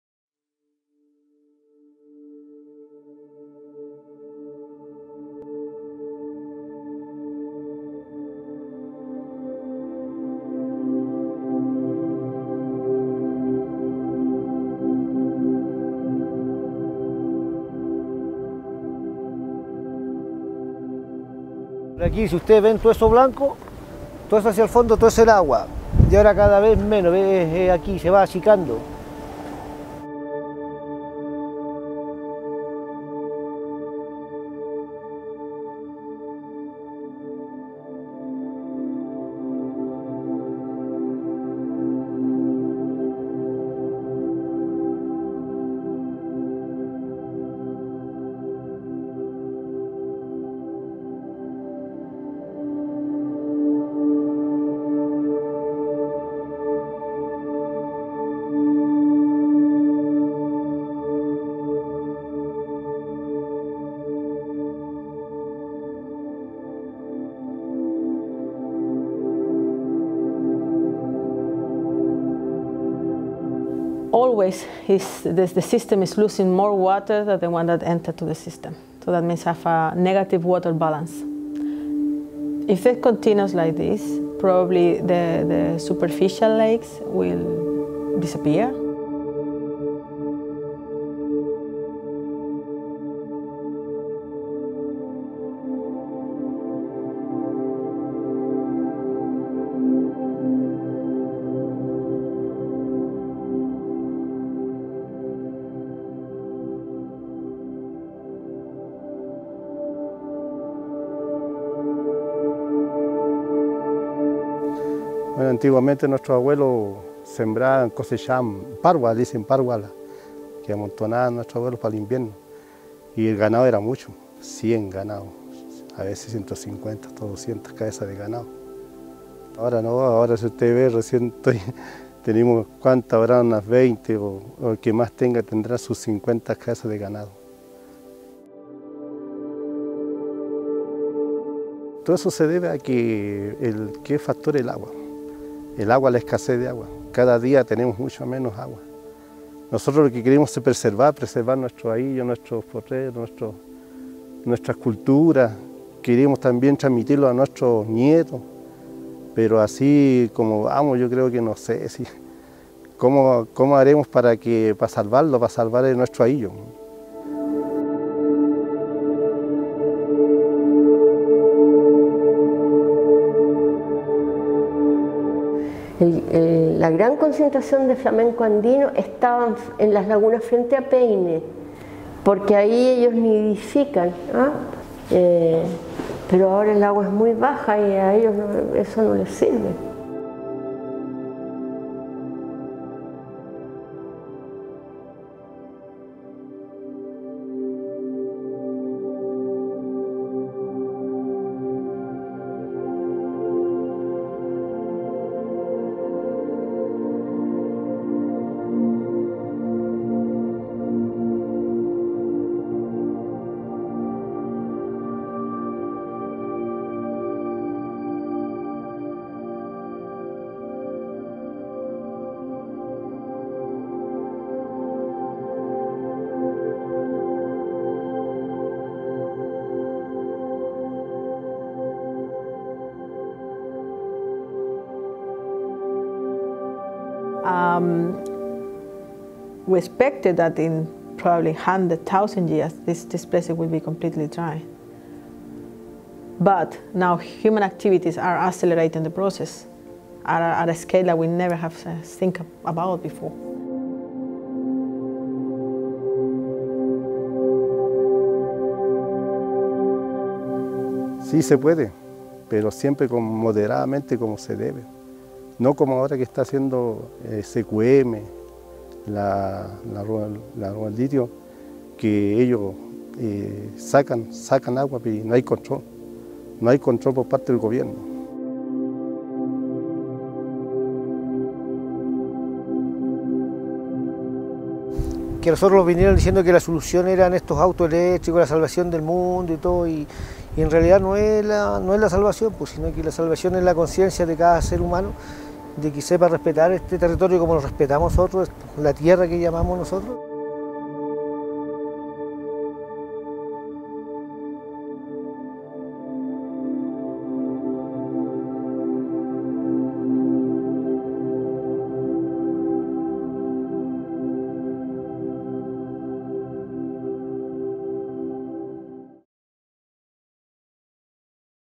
L'extracció de liti al desert de Xile, imprescindible per l'electrònica, provoca greus impactes.
Vídeo de la investigació periodística del nostre sòci europeu, Danwatch, en la qual és documenta que empreses com Samsung, Panasonic, Apple, Tesla i BMW reben bateries d'empreses que utilitzen liti Xile. Aquest mineral és essencial per a les bateries dels nostres telèfons, ordinadors, i els vehicles elèctrics que sovint és veuen clau per a una transició energètica verda.